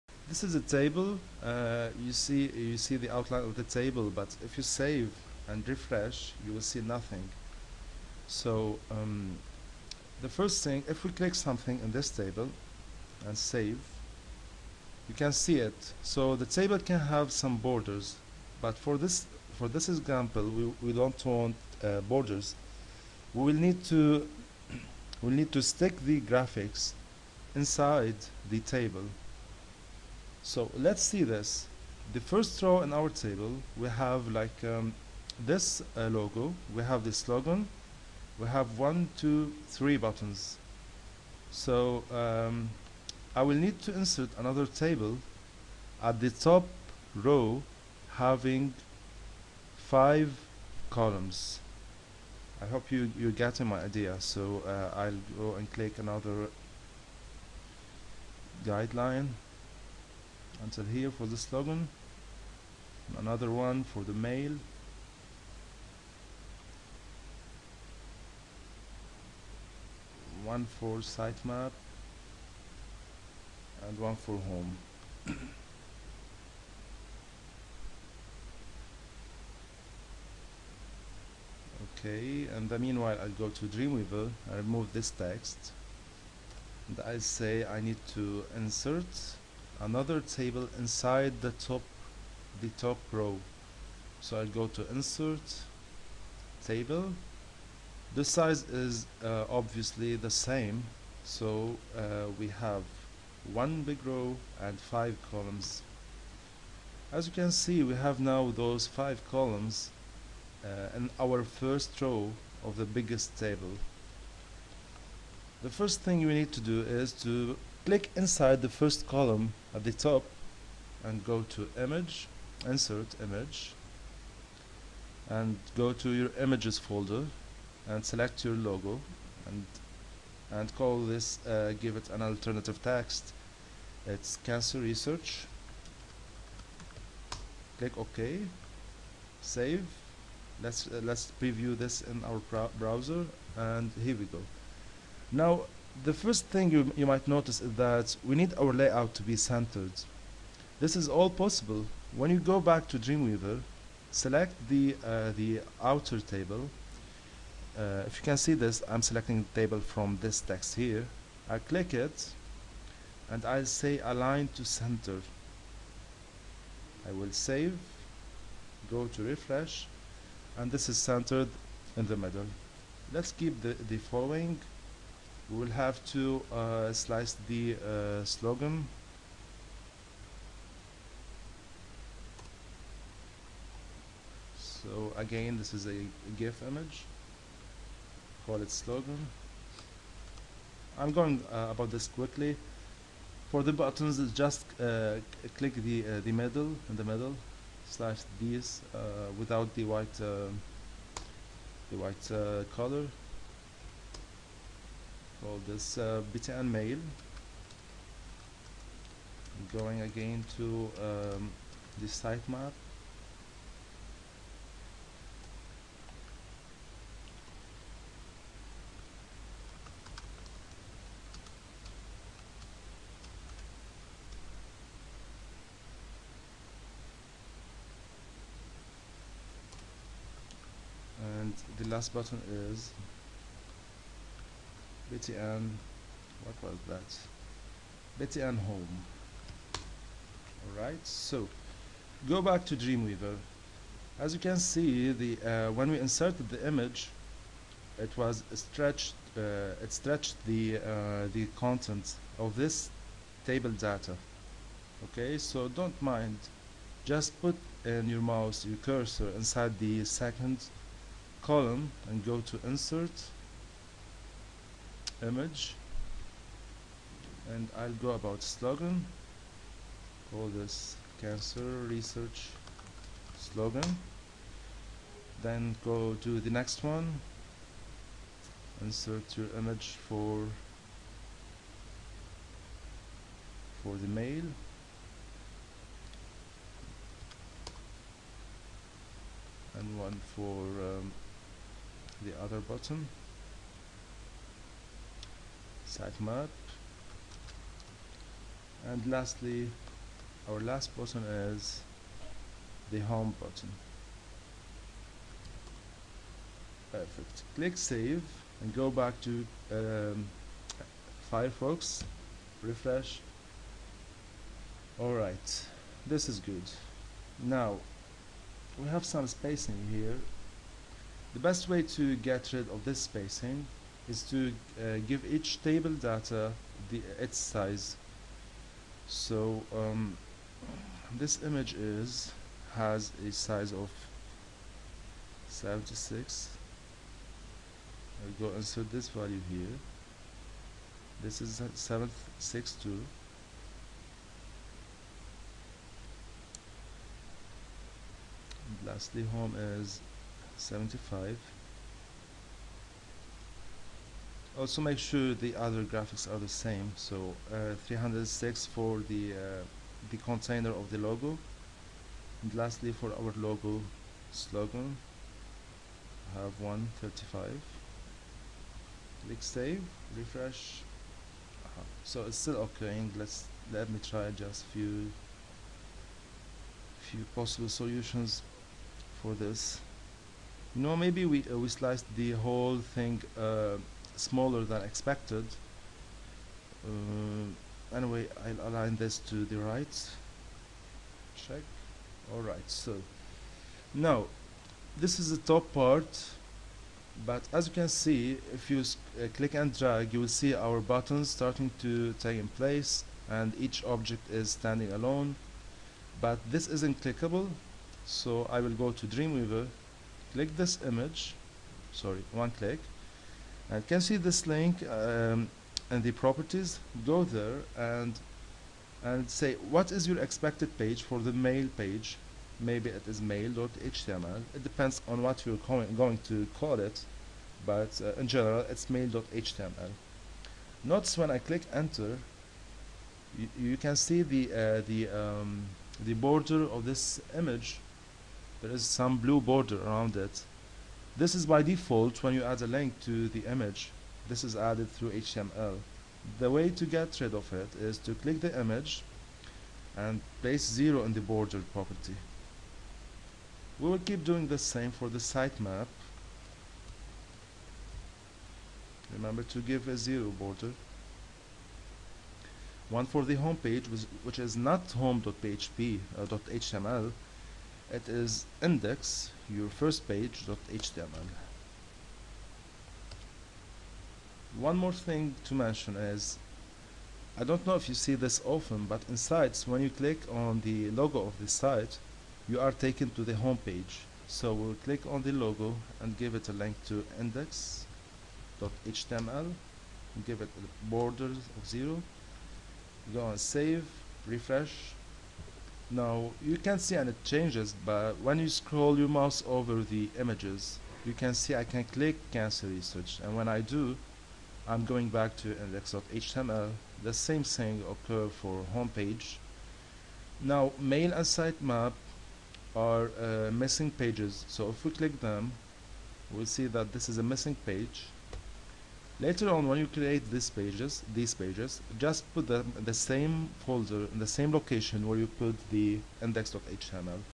Convert Photoshop to HTML - Part 2
This lesson is the extension of the first lesson about slicing your Photoshop document to an HTML website. The steps are easy to follow and when you finish viewing both videos, you are supposed to be able to manage most of PSD to HTML work using table-based method. More tips will come in future videos and soon I will cover Table-less slicing methods so make sure you come back soon.